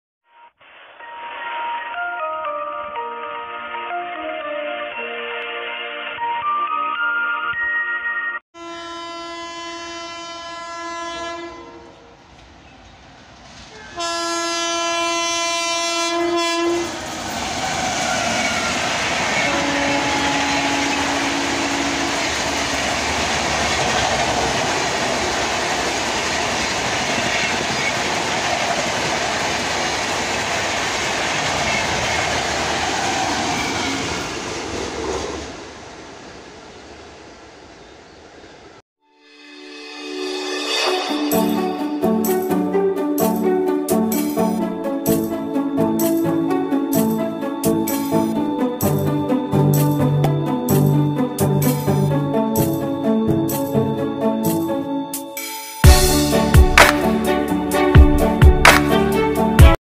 22222 Mumbai CSMT Rajdhani Express २२२२२ मुम्बई सीएसएमटी राजधानी एक्सप्रेस
#22222 Mumbai CSMT Rajdhani Express
H. Nizamuddin CSMT Mumbai
Via- Agra Cantt., Gwalior Junction, Virangana Lakshmibai Junction (Jhansi), Bhopal Junction, Jalgaon Junction, Nashik Road, Kalyan Junction

#२२२२२ मुम्बई सीएसएमटी राजधानी एक्सप्रेस
ह. निजामुद्दीन सीएसएमटी मुम्बई
वाया (के रास्ते)- आगरा छावनी, ग्वालियर जंक्शन, वीरांगना लक्ष्मीबाई जंक्शन (झांसी), भोपाल जंक्शन, जळगांव जंक्शन, नाशिक रोड, कल्याण जंक्शन

Train Video
Train Travel Video
Video